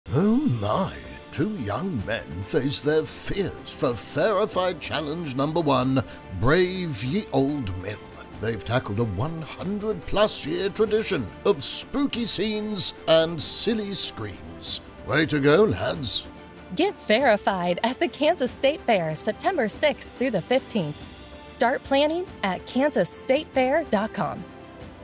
Fairified Challenge #1: Ye Old Mill
🚤✨ Are you ready to brave Fairified Challenge #1 – Ride the  Ye Old Mill? This iconic ride has been sending shivers up fairgoers spines for generations. Don't miss this true fair classic during the 2024 Kansas State Fair, Sept. 6-15 in Hutchinson!

Start planning your trip today at KansasStateFair.com.